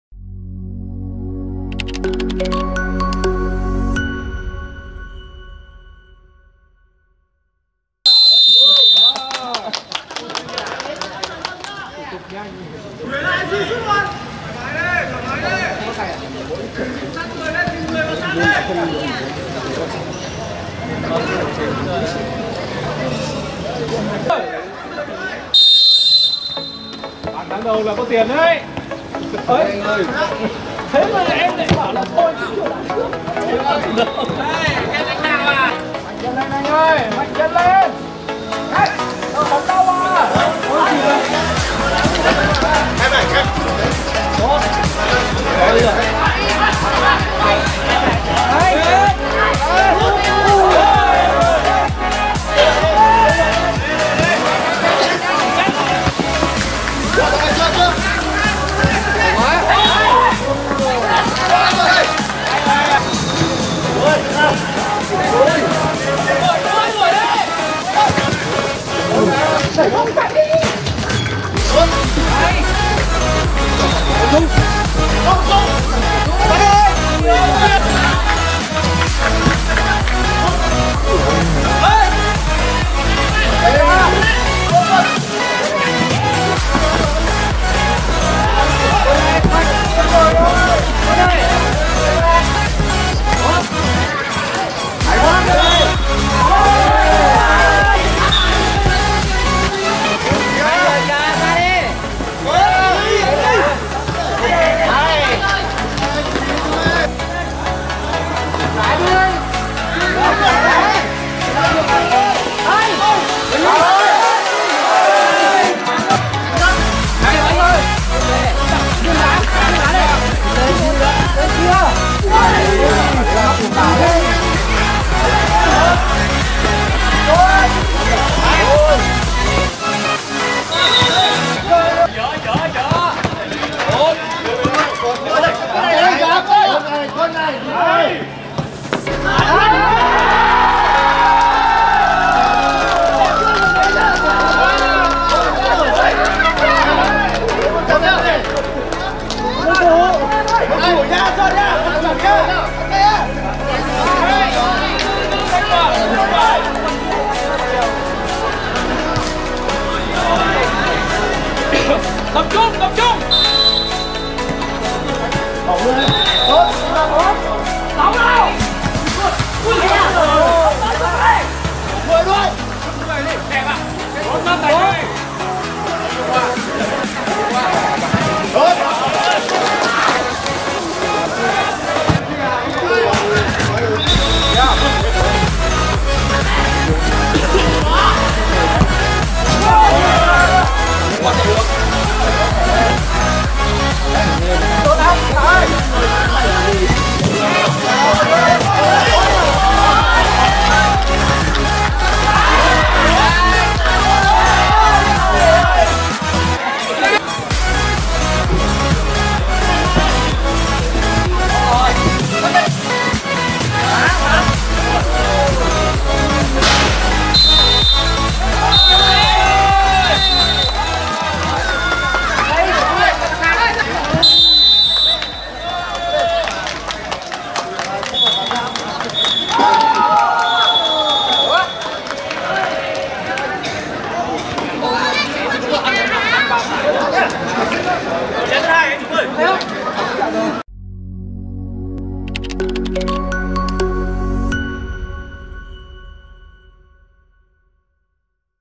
🎬 Highlights: OPENWAY FC -vs- IT - MB FC 🏆️ OPENWAY FRIENDSHIP CUP 2022
🎬 Highlights: OPENWAY FC -vs- IT - MB FC
🏆️ OPENWAY FRIENDSHIP CUP 2022
📅 Ngày 26/11/2022.
📍 Sân Bóng Đoan Môn - Trường Chinh - Hà Nội